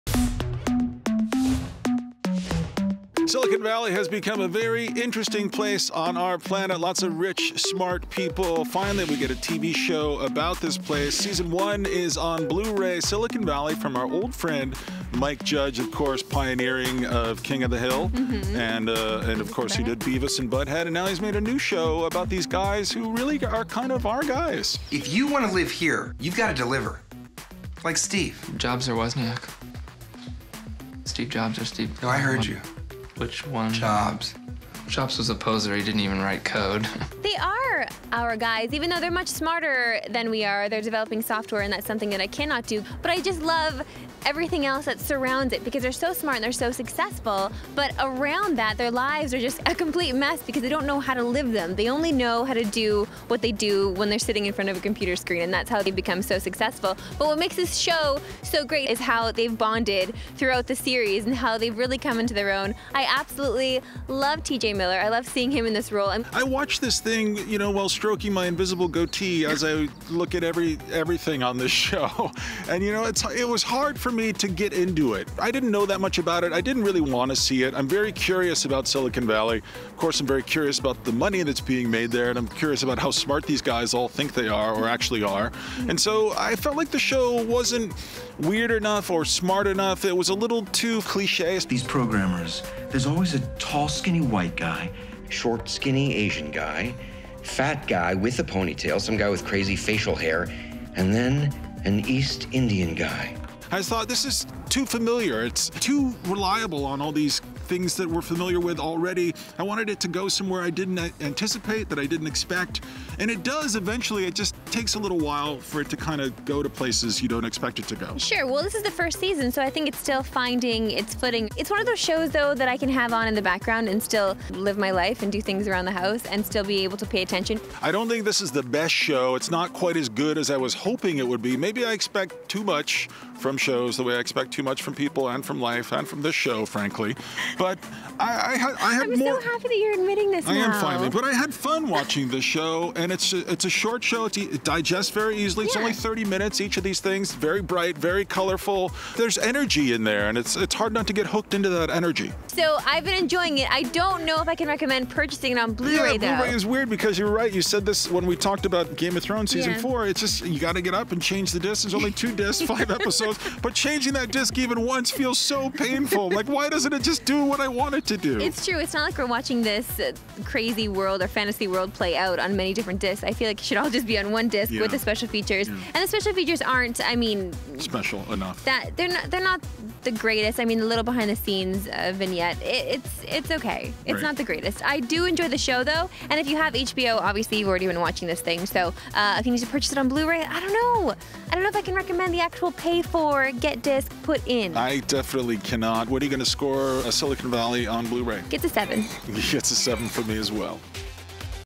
Silicon Valley: Season One Blu-ray Review
The first season of Mike Judge's tech comedy Silicon Valley has been uploaded to Blu-ray. Marissa and Scott let us know what they think.